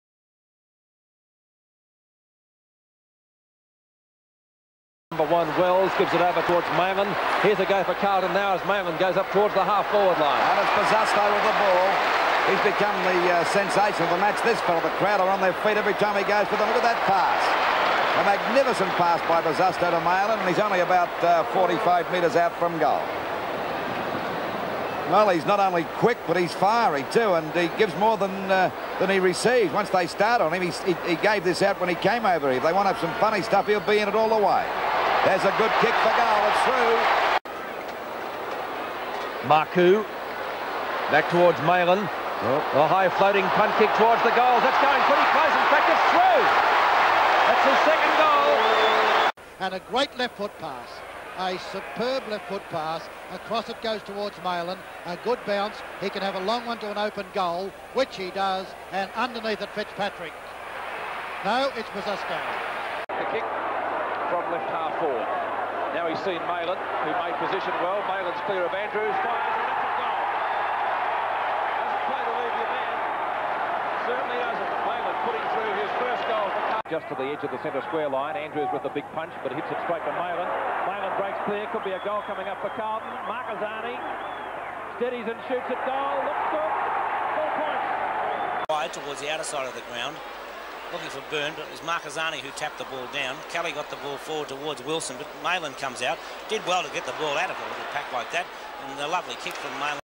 Phil Maylin 1981 - Carlton Football Club Past Player
Vision from Phil Maylin's 1981 VFL season for the Carlton Football Club.